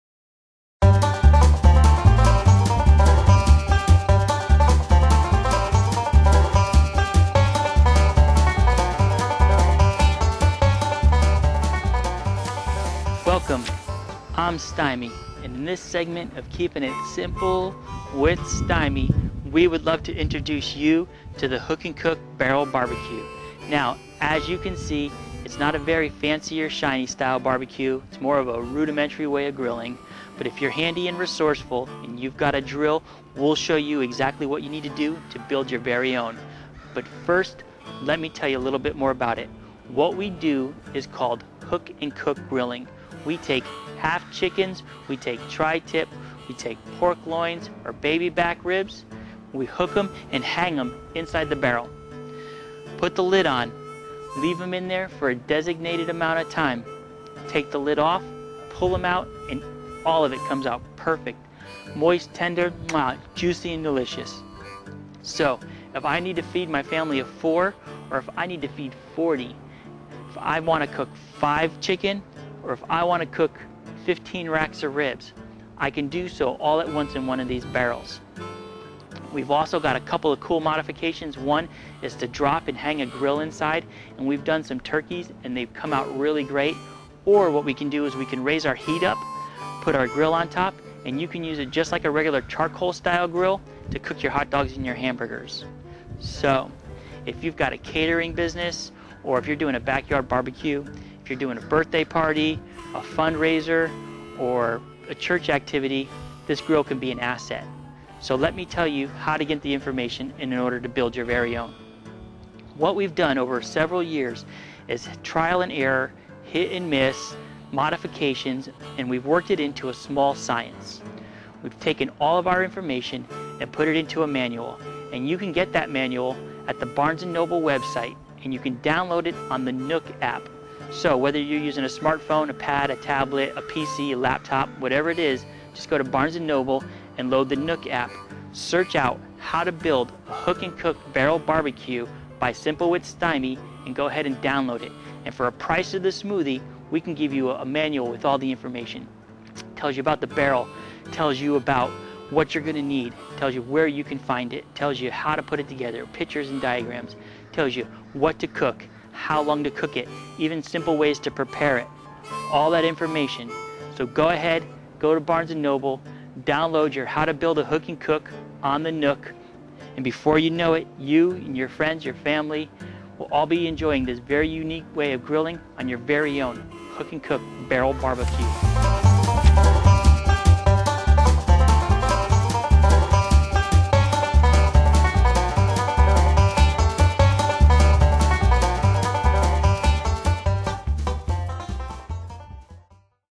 UDS How to Build & What is a Hook-n-Cook Barrel BBQ
link for ebook This Video will give you an overview of the worlds Best Barrel BBQ.